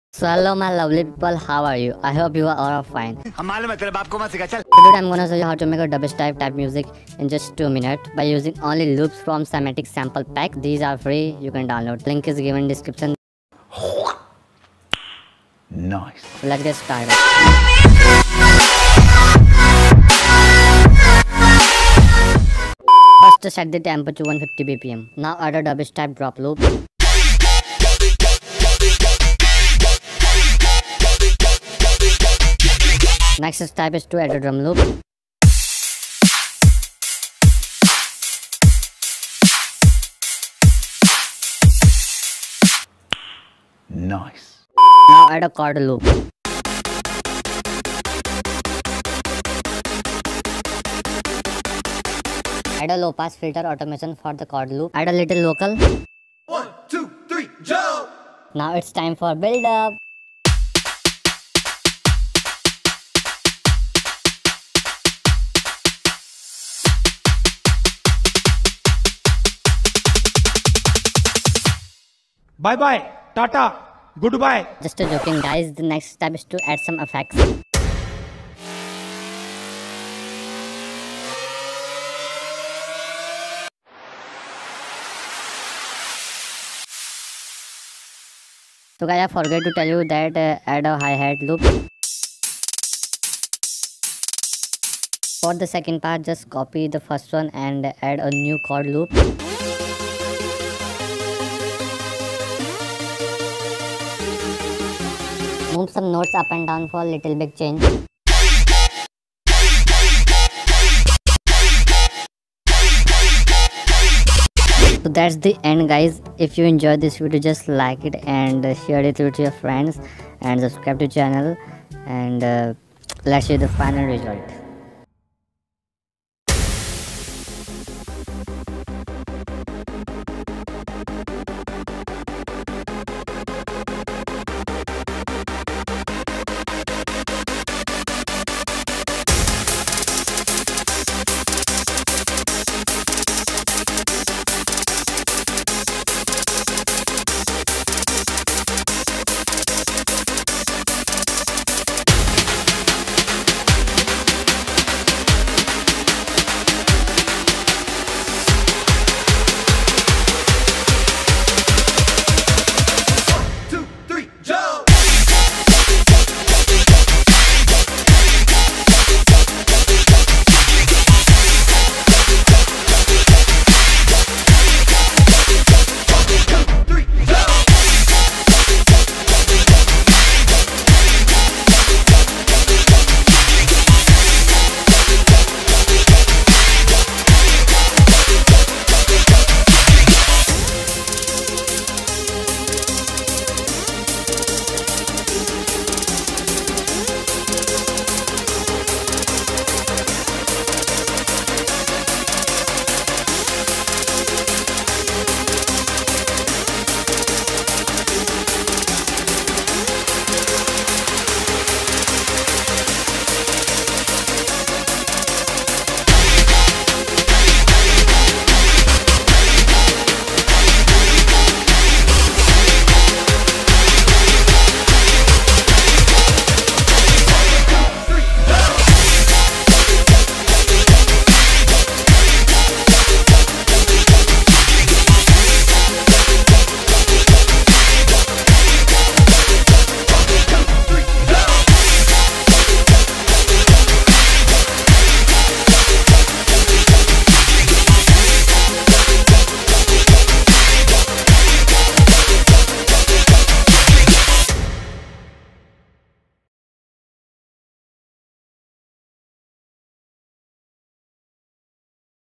How to dubstep in 2 minutes | lmms tutorial #3
hey guys...
I'm emagico and I'm gonna show u how to make a dubstep track in lmms...
i know it's not too good but i tried my best..

dubstep tutorial
how to dubstep
dubstep tutorial in lmms